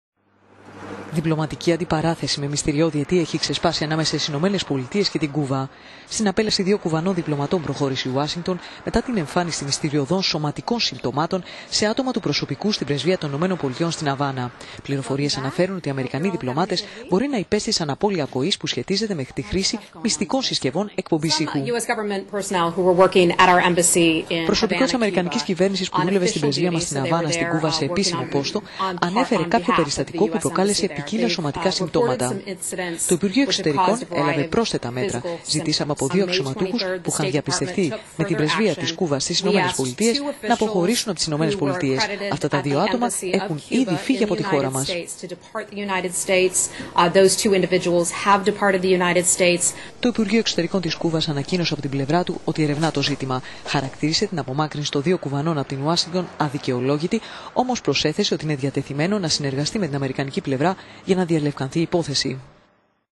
Στην απέλαση δύο Κουβανών διπλωματών προχώρησε η Ουάσιγκτον
Διπλωματική αντιπαράθεση  με μυστηριώδη αιτία έχει ξεσπάσει ανάμεσα στις Ηνωμένες Πολιτείες και την Κούβα. Στην απέλαση δύο Κουβανών διπλωματών προχώρησε η Ουάσινγκτον , μετά την εμφάνιση «μυστηριωδών σωματικών συμπτωμάτων» σε άτομα του προσωπικού στην Πρεσβεία των ΗΠΑ στην Αβάνα. Πληροφορίες αναφέρουν ότι οι Αμερικανοί διπλωμάτες μπορεί να υπέστησαν απώλεια ακοής που σχετίζεται με τη χρήση μυστικών συσκευών εκπομπής ήχου. Σύμφωνα με την Χέδερ Νάουερτ, εκπρόσωπο τύπου του State Department:«Προσω…